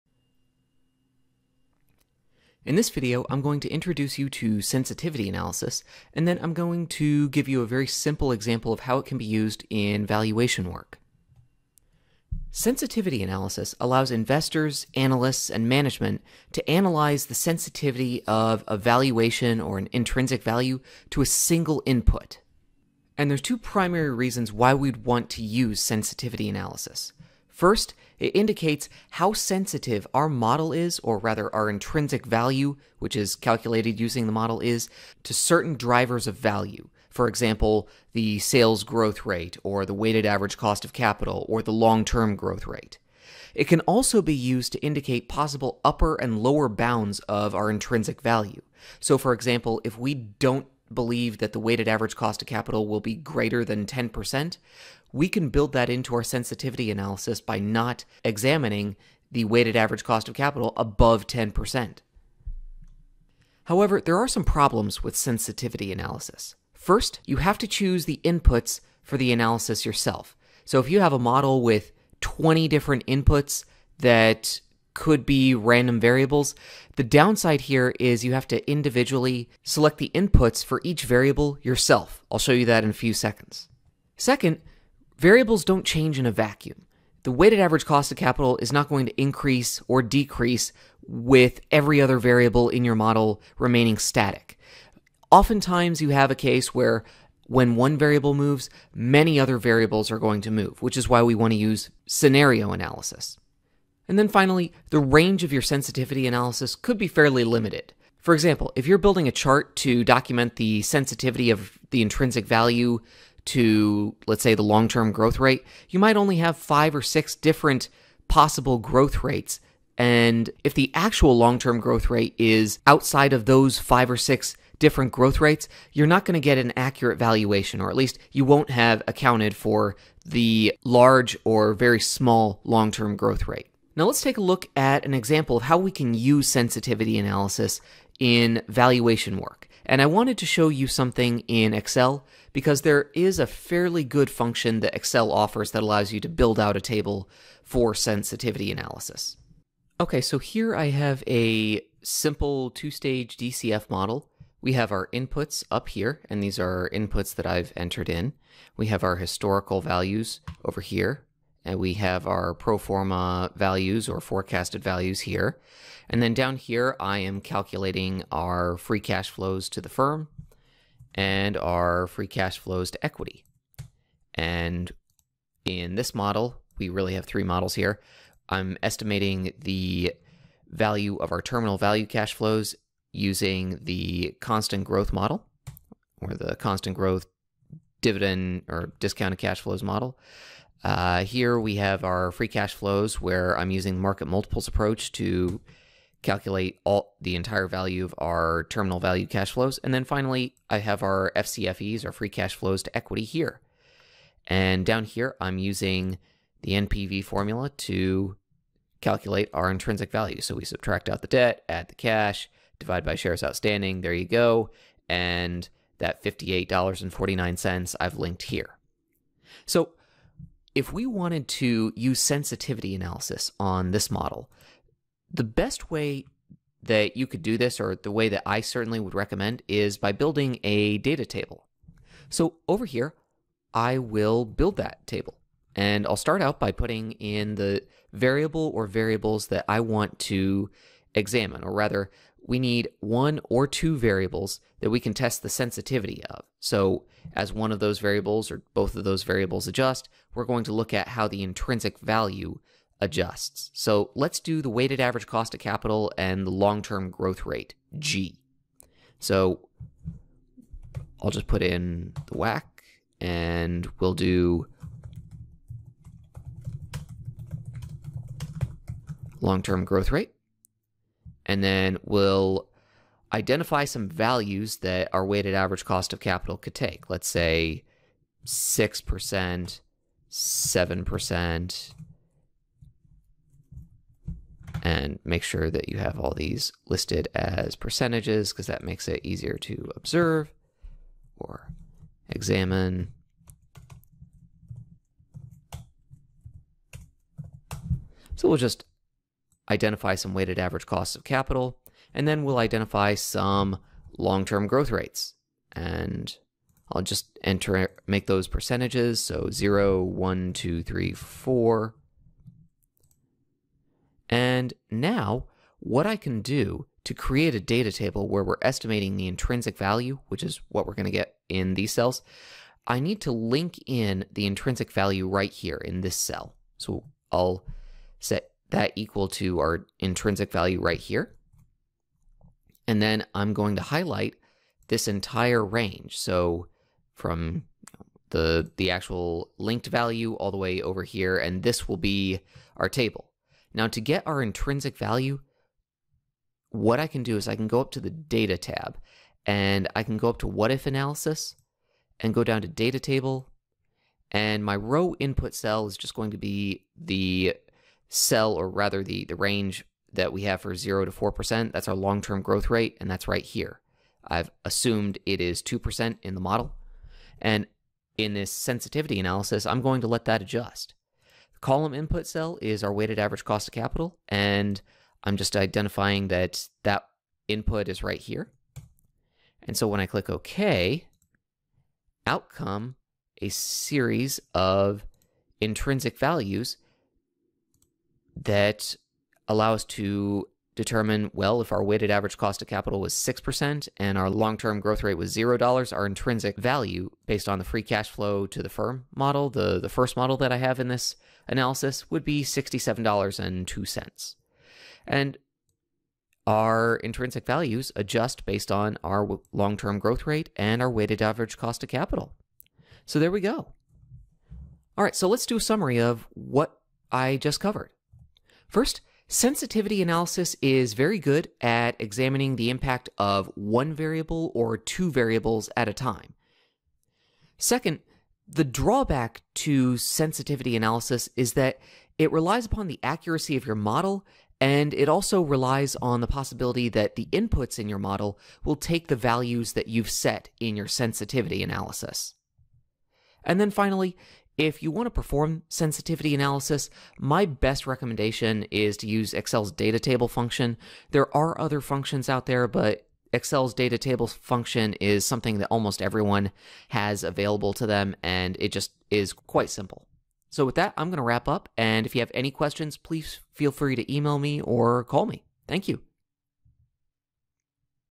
Sensitivity Analysis for Equity Valuation
In this video, I briefly discuss the pros and cons of sensitivity analysis.  I then provide a quick example of sensitivity analysis in Excel using the Data Table function.